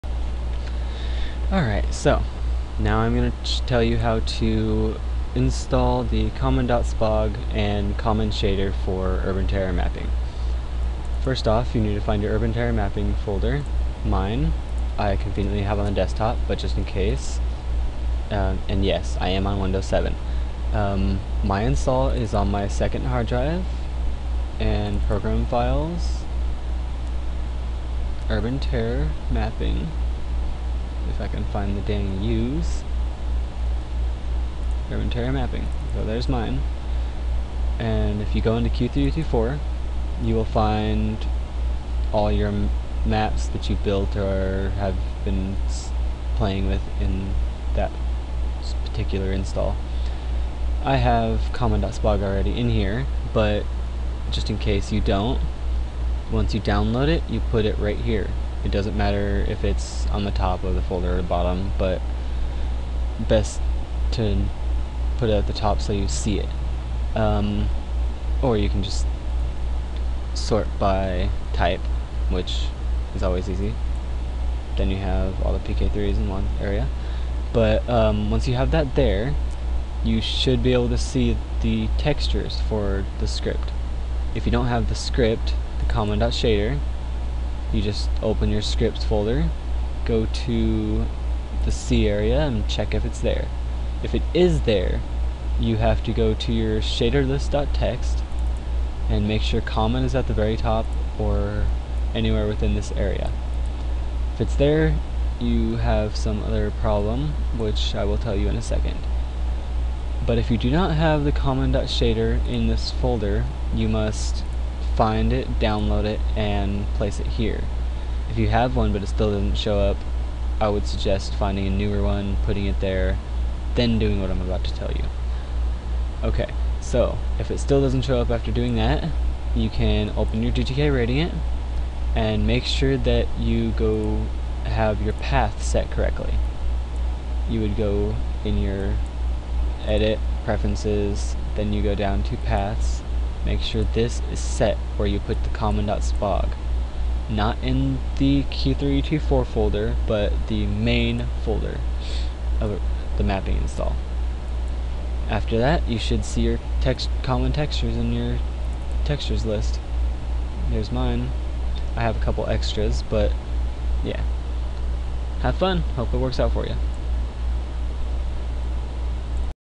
GTKradiant and common-spog -urbanterror specific-
this is just to show you a basic of where and how to install the common-spog and common.shader for urbanterror mapping.
download for the common-spog, urbanterror.def, shaderlist.txt, and the common.shader.
the urbanterror.def file included is an updated one witht he "haxy weapon insertion " definitions for urbanterror

a comment i posted to someones website after they asked me a question that i thought others would get some help from!
you just need to get the settings right, the team deathmatch and FFA spawn points are there in GTKradiant ( if you put the urbanterror.def file in there right, i for one put it everywhere i could think of, even the .q3 folder of GTK radiant just to make sure.) then in GTKradiant you have to right click on the grid, find "info_player_start" for the FFA spawn points and the "info_ut_spawn" ( if i remember correctly) for the team spawns, BUT you must set the teams of those   ( key = team        value = red or blue )